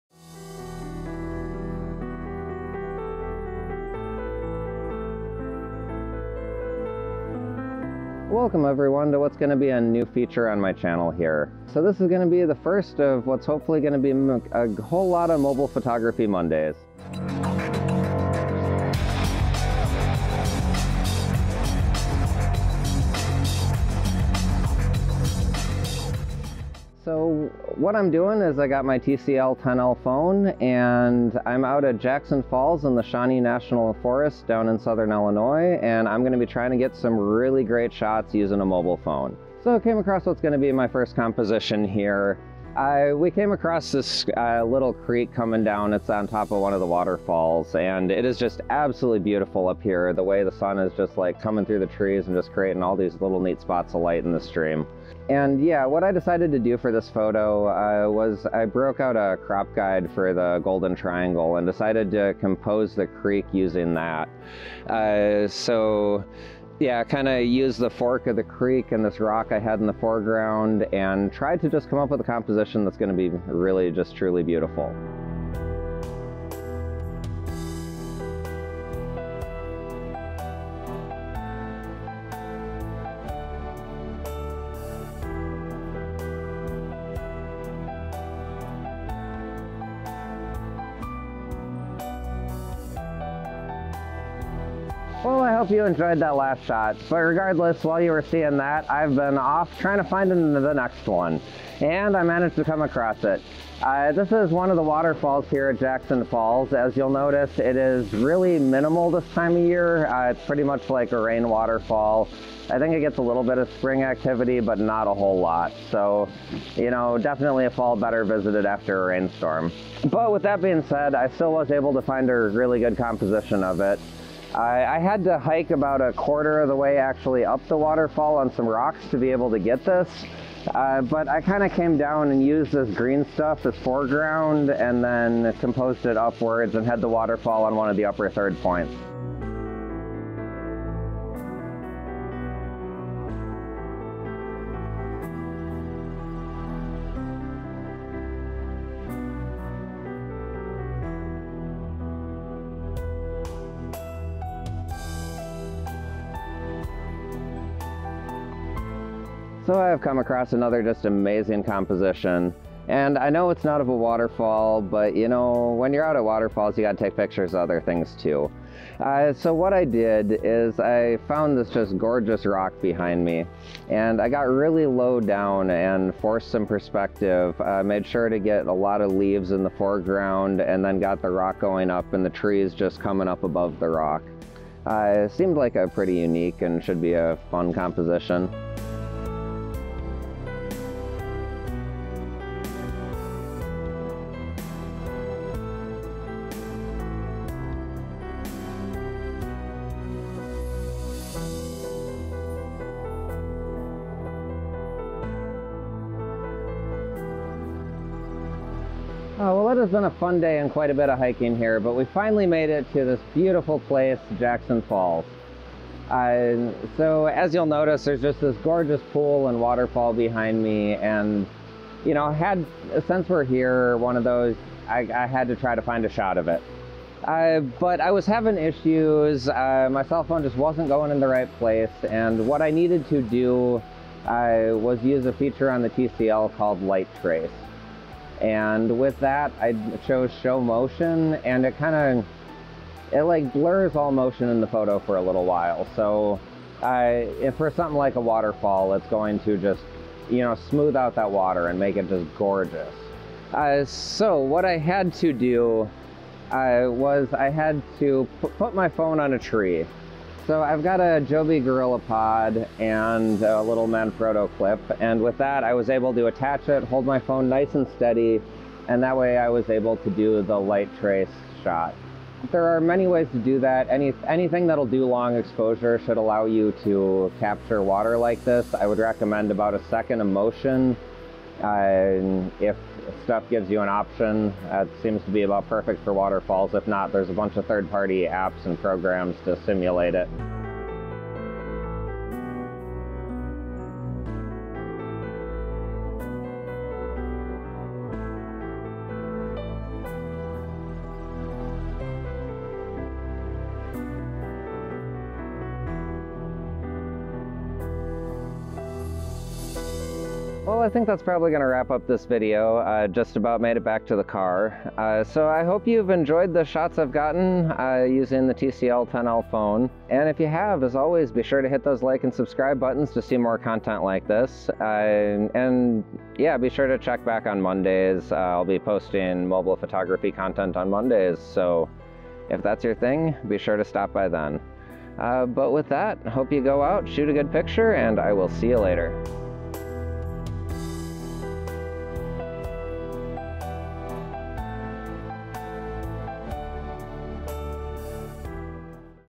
Jackson Falls, Illinois | Mobile Photography Monday (TCL-10L)
Join me at Jackson Falls for some beautiful mobile photography! All images were shot with the TCL-10L phone and edited with Lightroom Classic. Enjoy the journey around this unique location in the Shawnee National Forest of Illinois. This video is also filled with mobile landscape photography tips to help out on your adventures :)

Video was filmed with:
Canon M50

MY OTHER PROJECTS:
Galaita (my nature photography channel)